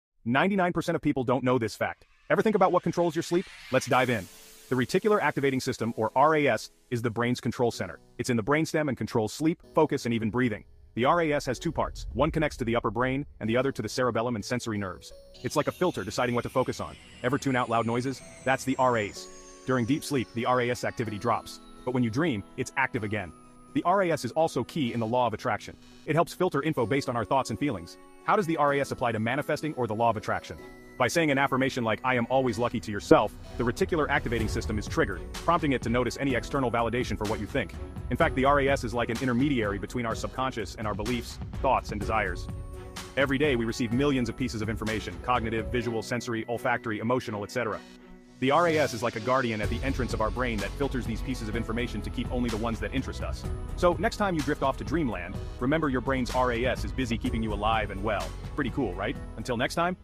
Unlocking the Brain (The Power of the Reticular Activating System)
Discover the vital role of the Reticular Activating System (RAS) in controlling your sleep, consciousness, and sensory perception. Learn how this brainstem network impacts your daily life and dreams in our in-depth exploration of the RAS.